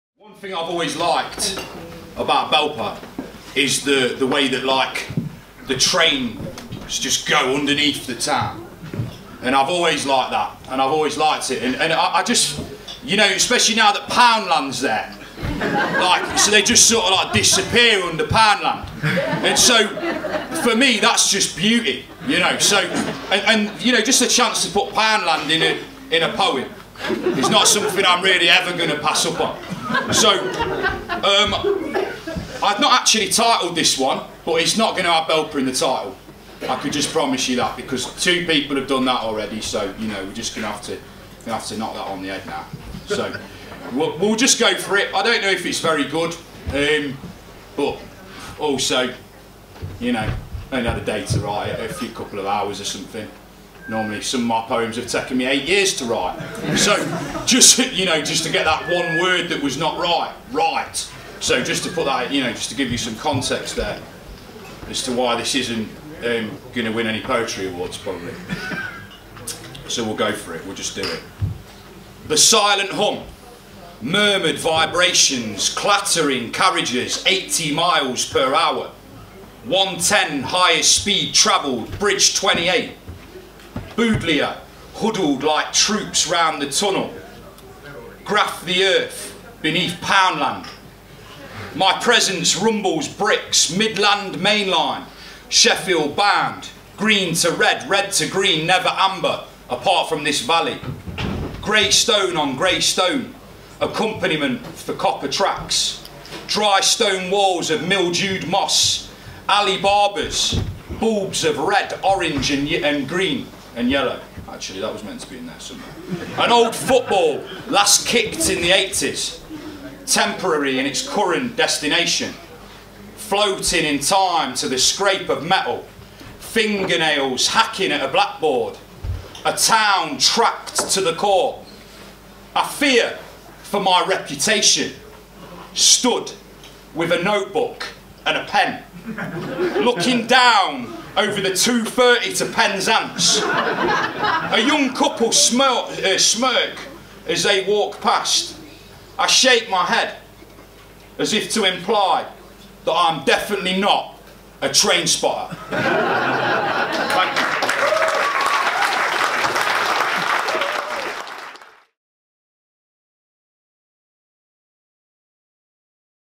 Jamie Thrasivoulou - Belper Poem (The Travelling Band Project)
The Travelling Band Project took songwriters on a creative tour of Derbyshire towns to create new music and perform on the same day.